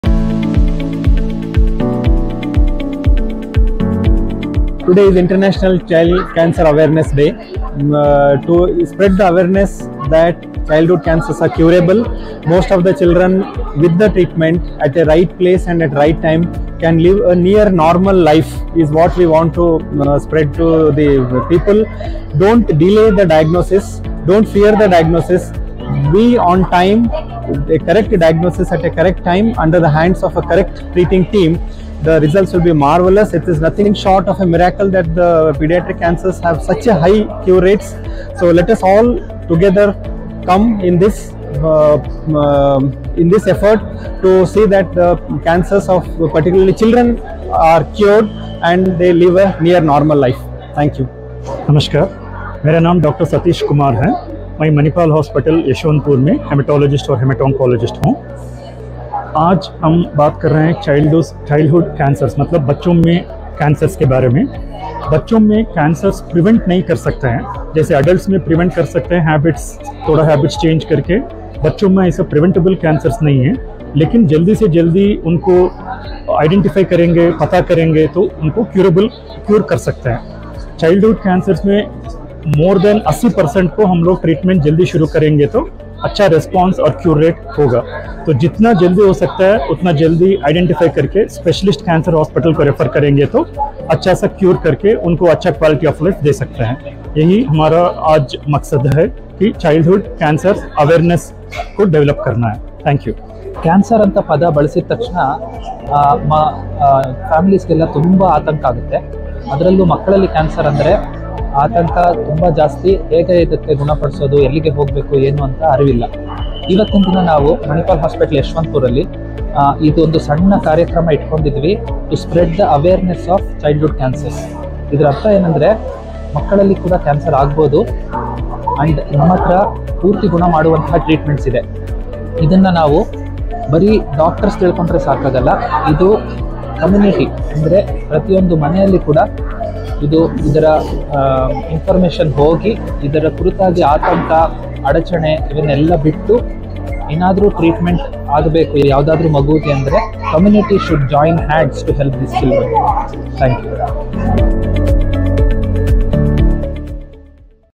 Childhood Cancer Day | Cancer Hospital | A Journey of Optimism at Manipal Hospital Yeshwanthpur
On the occasion of Childhood Cancer Day, Manipal Hospital Yeshwanthpur organised a special event in the honour of childhood cancer patients. Dr. Neelesh Reddy, Consultant - Medical Oncology, Dr. Satish Kumar A, Consultant - Hematology and Hemato-Oncology, and Dr. Vinay Munikoty Venkatesh, Consultant - Paediatric Haematology, Oncology & BMT, share medical insights. Young cancer survivors share some inspiring words on the journey from diagnosis to treatment in this video. Watch to know more about the event.